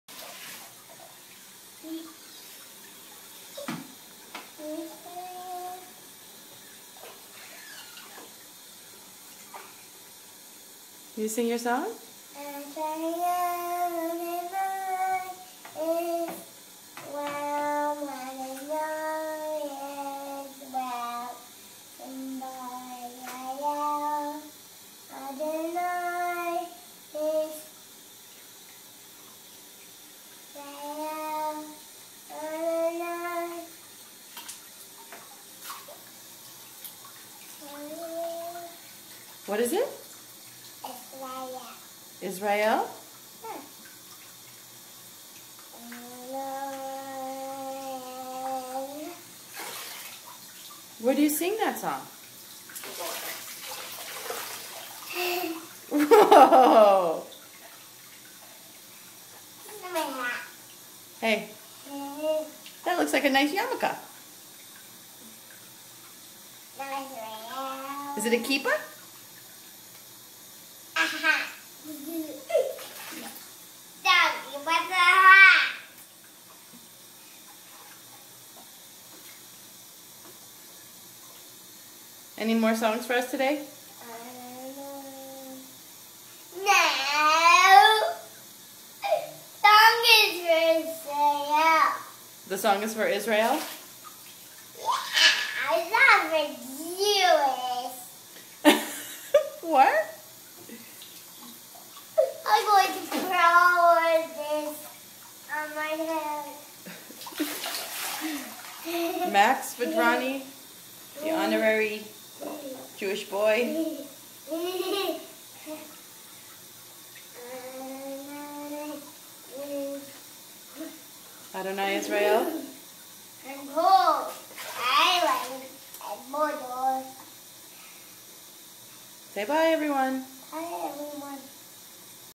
Max's Hebrew
Max singing about the "Jewish holiday" as he told me